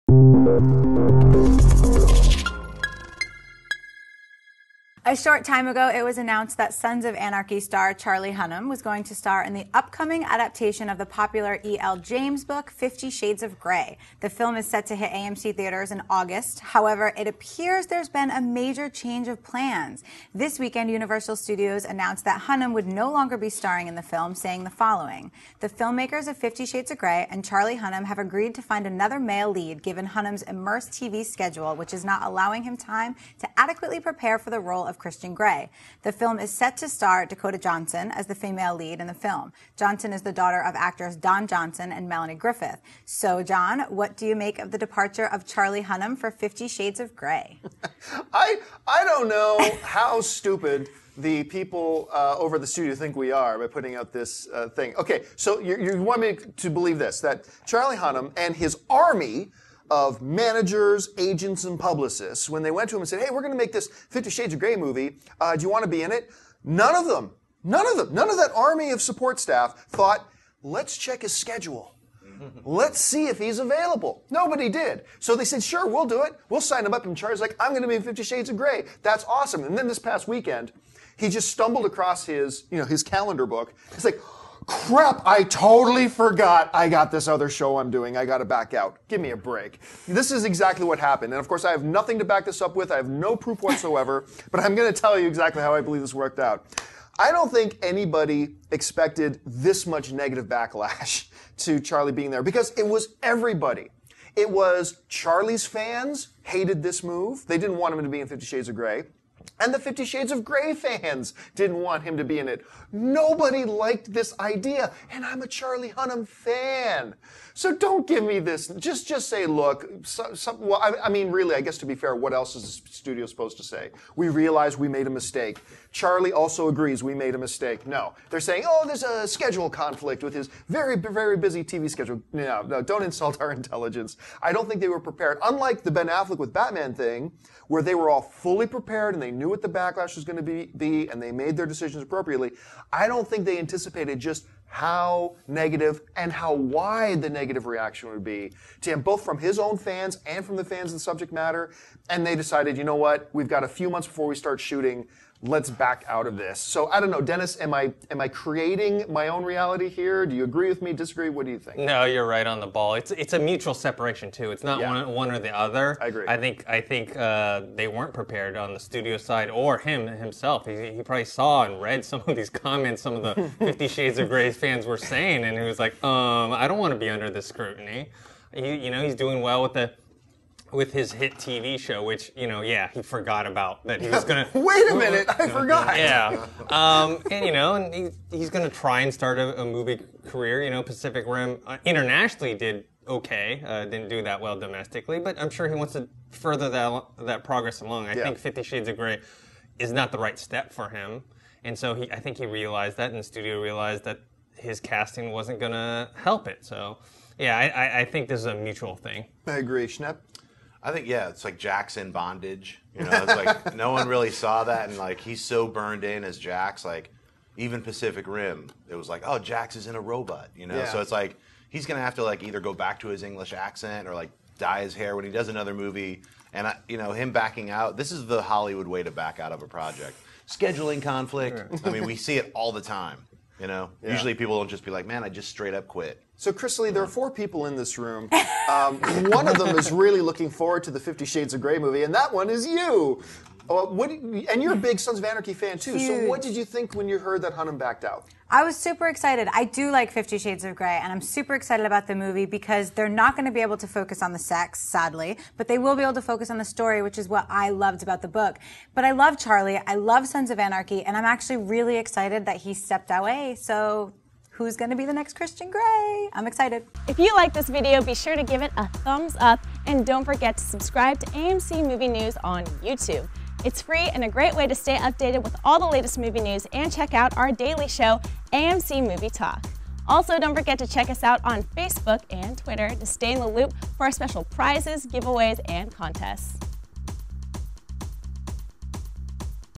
Charlie Hunnam Walks Away From 50 SHADES OF GREY - AMC Movie News
This weekend Universal studios announced that Hunnam would no longer be starring in the film 50 Shades Of Grey.